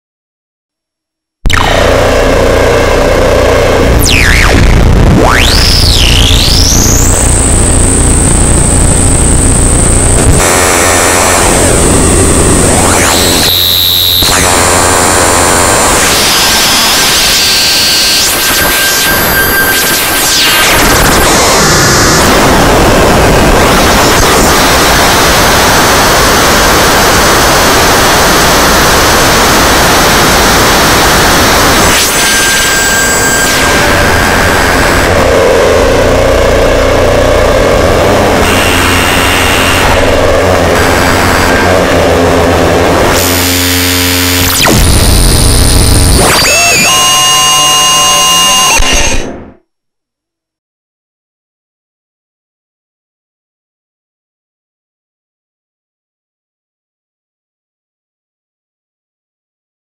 RR-151224-A
A short noise improvisation with my RVO… With a pinch of visual digital distortion !